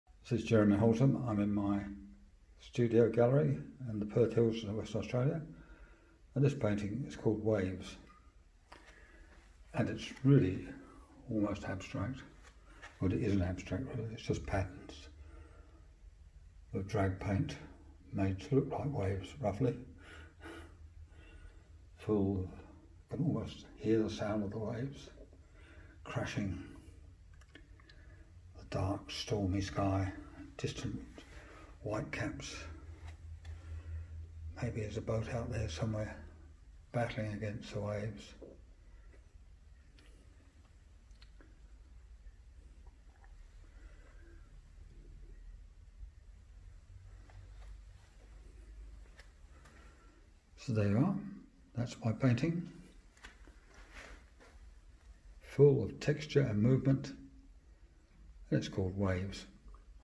1129 Waves
40 by 51 cm painting oil on canvas - I tried to capture the movement and sounds of crashing waves pounding a beach in a dark storm.  To achieve this effect I used  Winsor and Newton alkyd oil paints dragged with colour shapers